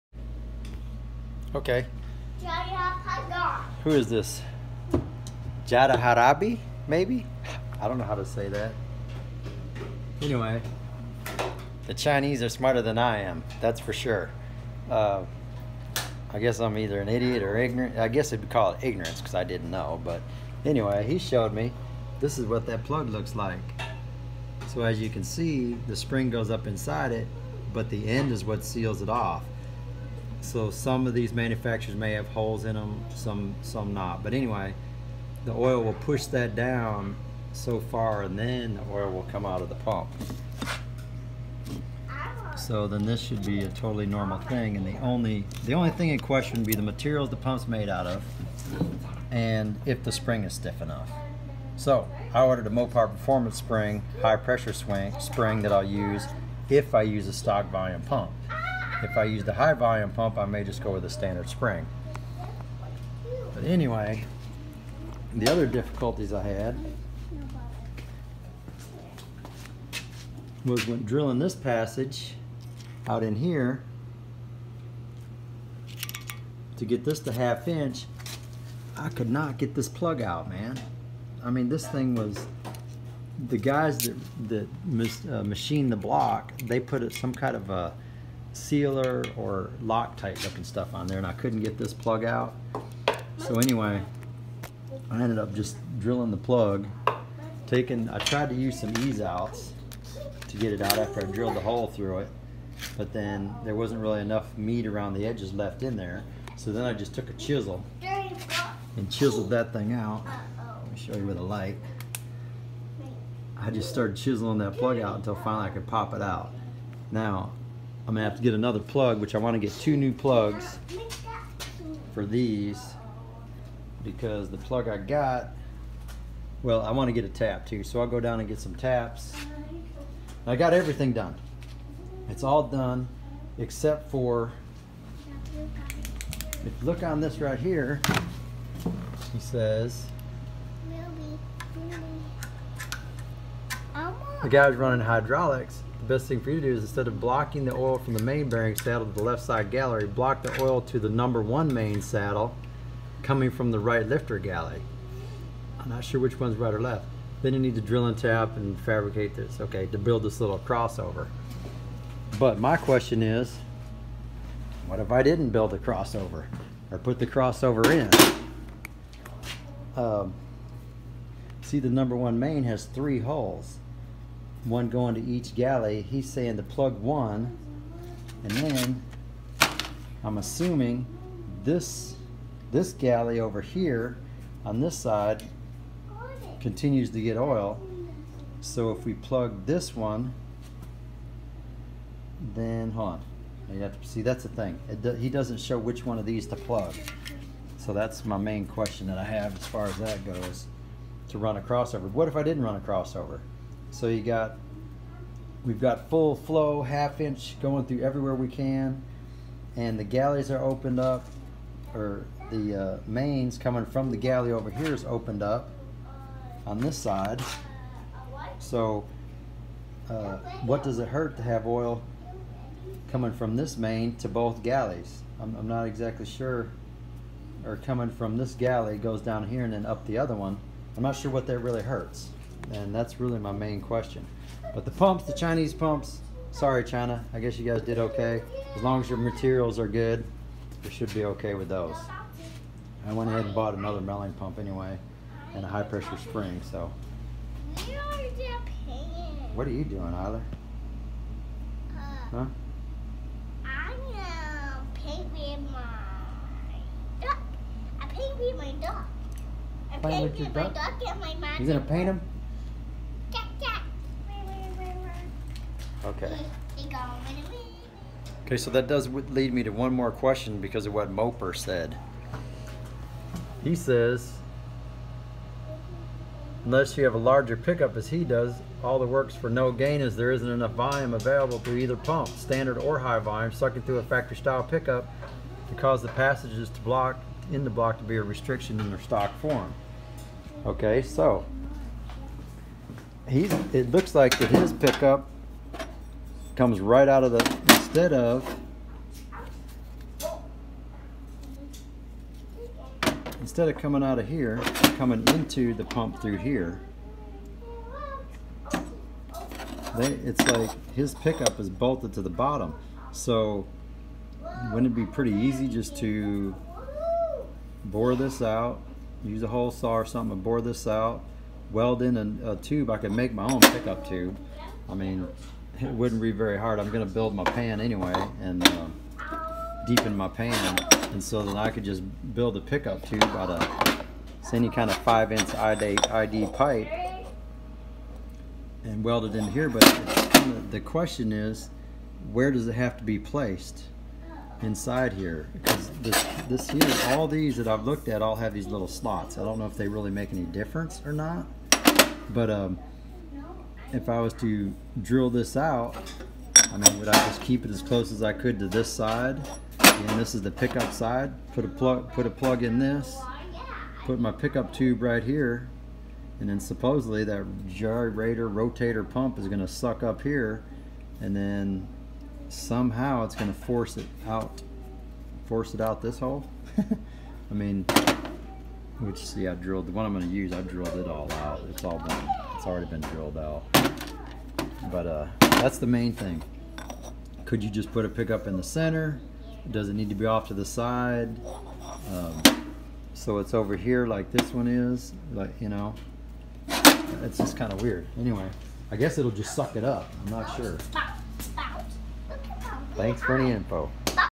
China ain’t that bad. :-) $$
Maybe I should start trading their currency.
lol
Couple more questions on the small block mopar oiling mods.
Showed a little more progress on drilling.